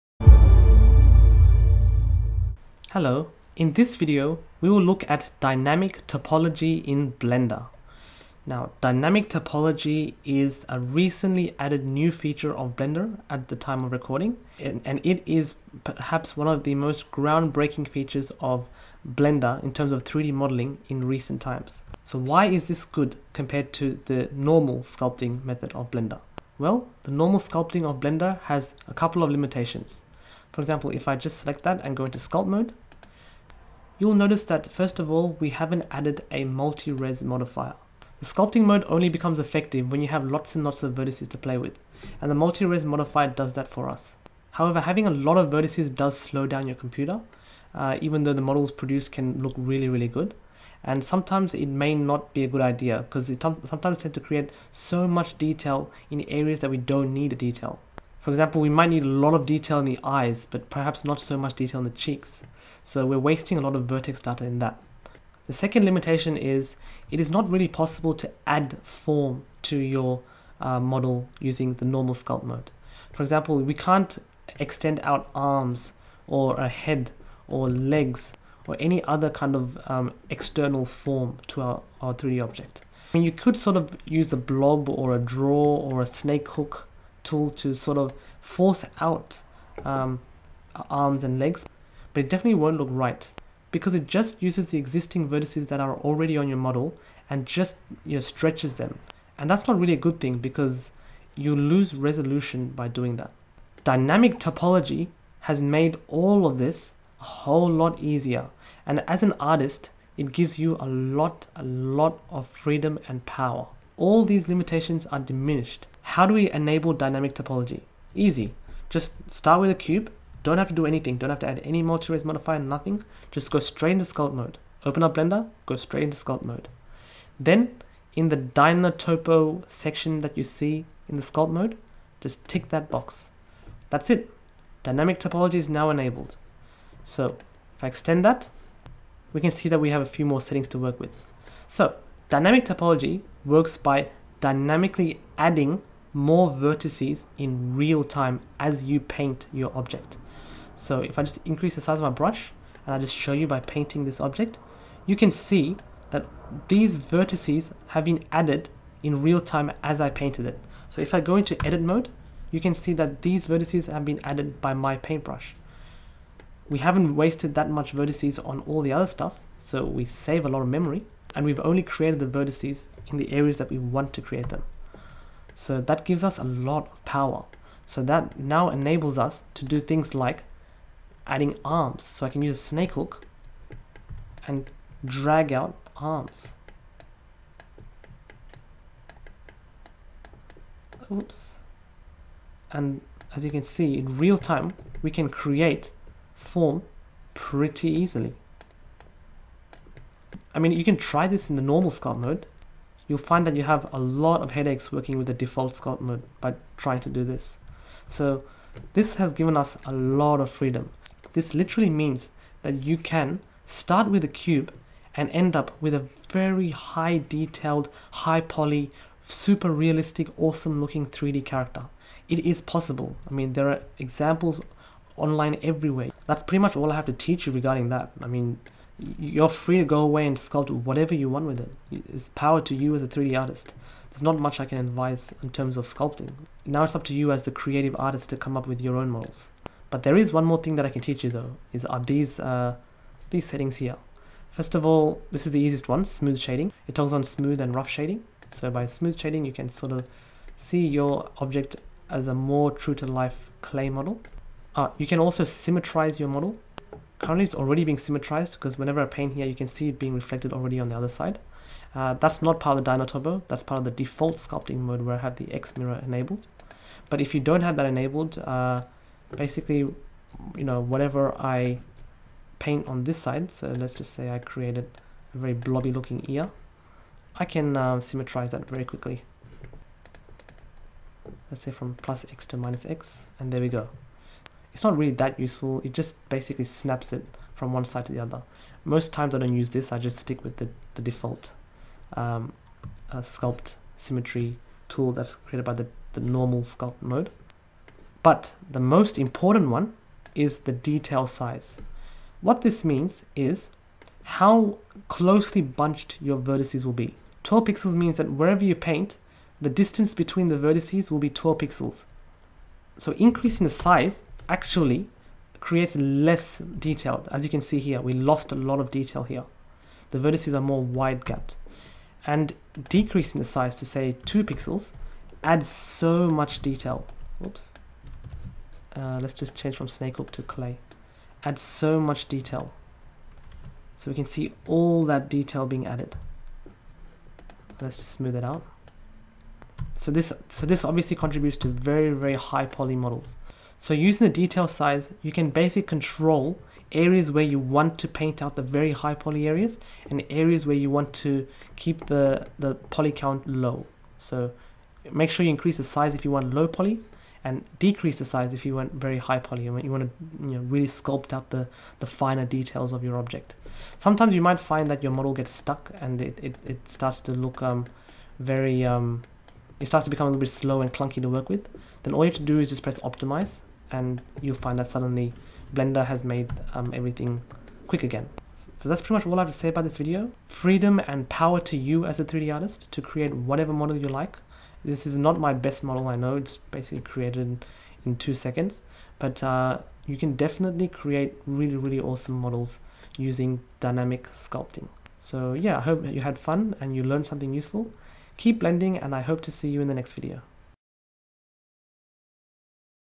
Dynamic Sculpting in Blender (Beginner Tutorial)
In this video, you will learn about DynaTopo, and how it is FAR better and more powerful than the normal sculpting method of Blender. With DynaTopo, you, the artist, have power and freedom to create and shape the models that you have dreamed of!

This video is part of the Introduction to Blender For Beginners Series by Thilakanathan Studios!